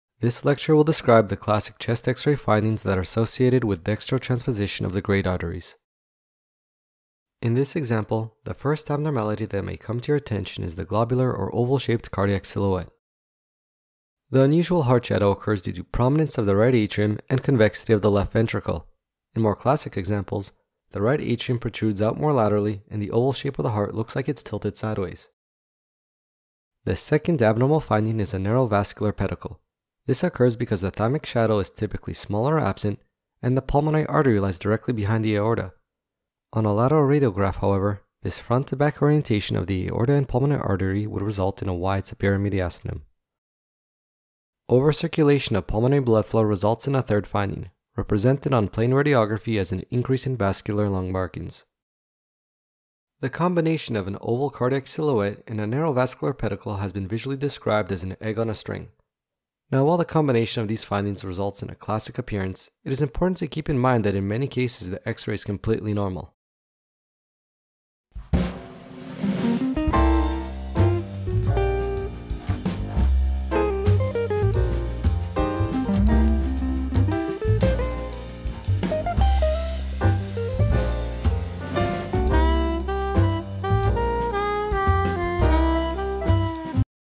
Transposition of the Great Arteries: Explanation of Chest X-ray Findings (Egg-on-a-string)
The combination of an oval cardiac silhouette and a narrow vascular pedicle has been visually described as an egg-on-a-string. In cases with greater degrees of cardiomegaly, the term egg-on-a-side has been used. Now while the combination of these findings result in the classic appearance, it is important to keep in mind that in many cases the x-ray is completely normal.